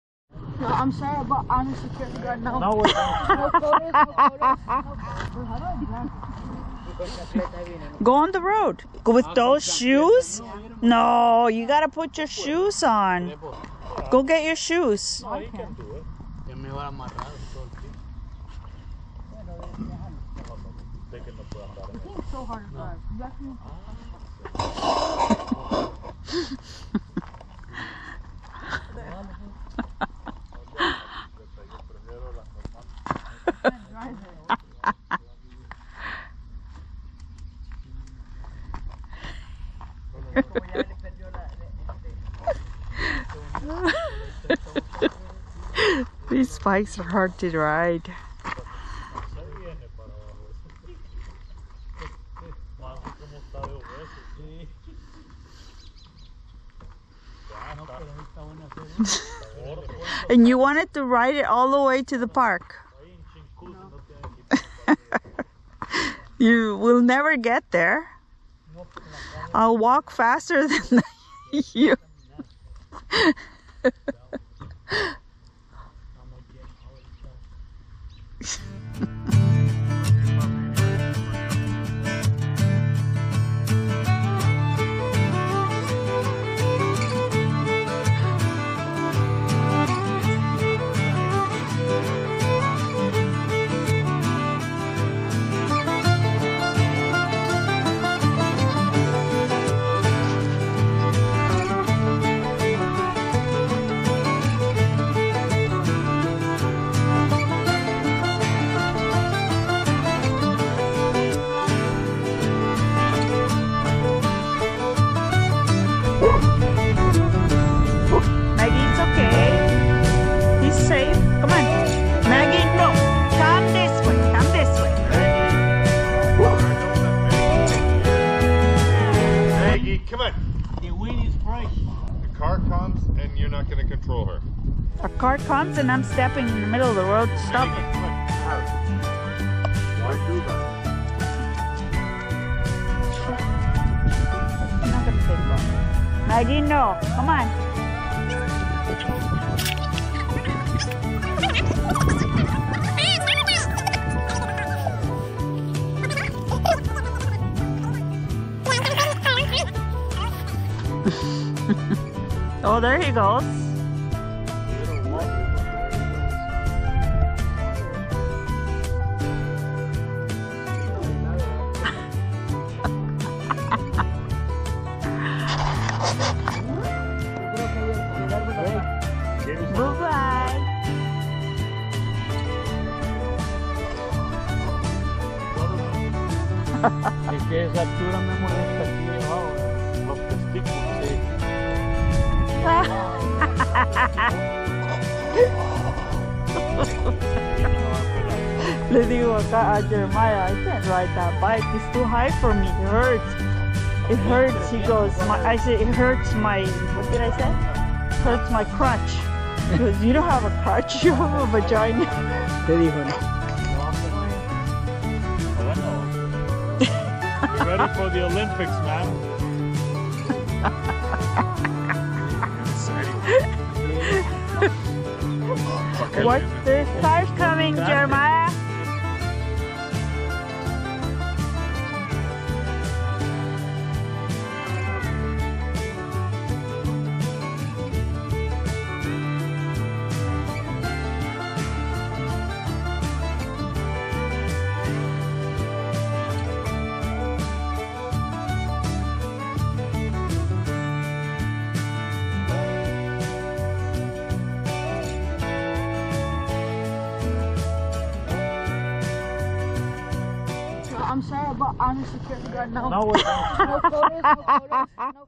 Kids say the darndest things!
A fun moment with family learning to ride a recumbent bike LOL